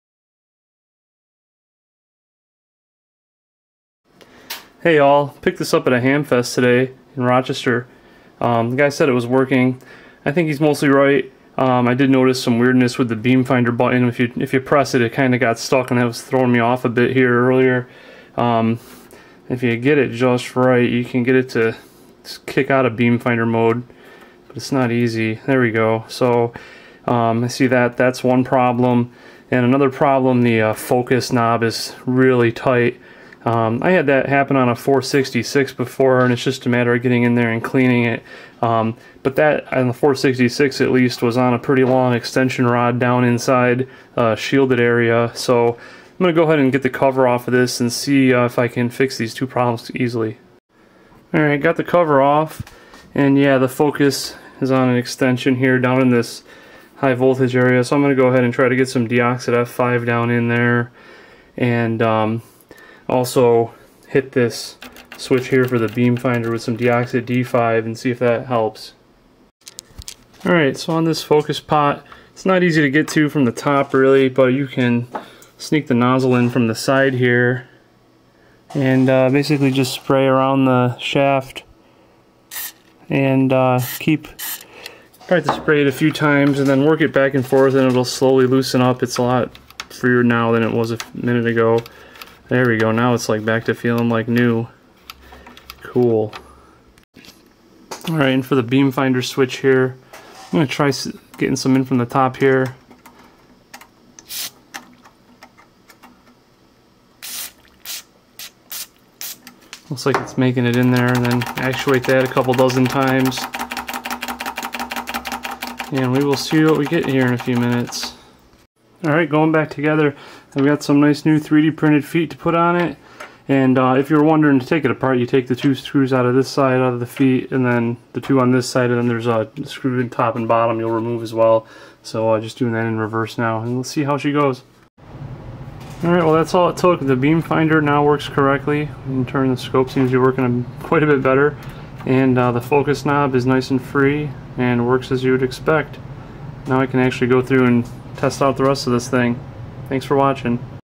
Tektronix 475 Oscilloscope Hamfest Find - Quick Fix
I found this at the Rochester Hamfest this morning, the owner had listed it for sale in working condition but it gave me issues when I plugged it in.  Thankfully they were easy to resolve with a little bit of Deoxit.